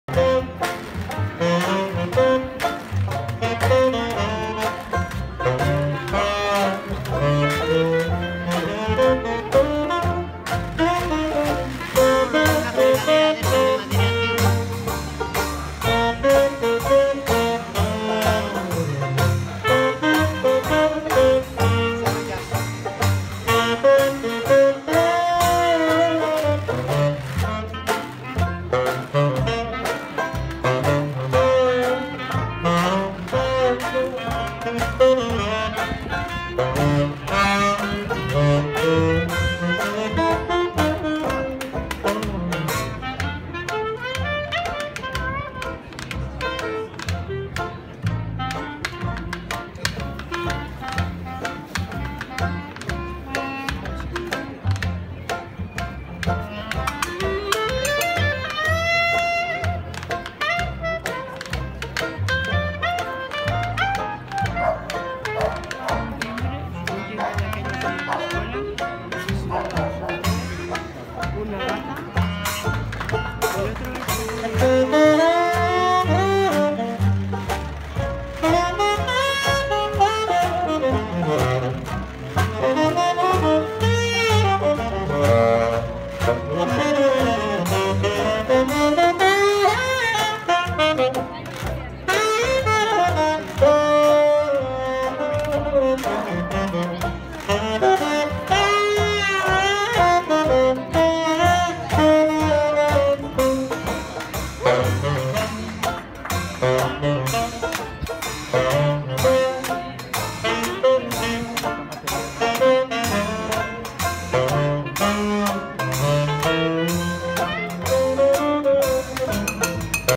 Jazz en la Calle Embajadores.
Un día de en el Rastro de Madrid te da alegrías como esta

Encontrar artistas así en plena calle, las personas se quedan encantadas, disfrutando de su trabajo y cuando acaban la canción algunos salen en desbandada para evitarse pagar en el mejor de los casos para ellos un € o dos.
Y no pensamos en la alegría del buen rollo que nos dan.
Desde somos partidarios de disfrutar de la música si, pero agradecérselo con un poco más de un aplauso, por qué los artistas también tienen que pagar facturas.
La música nos hace volar y nos hace bailar, nos hace reír y viajar.
No hace falta irse muy lejos para disfrutar a diario, lee, canta y viaja con la  música.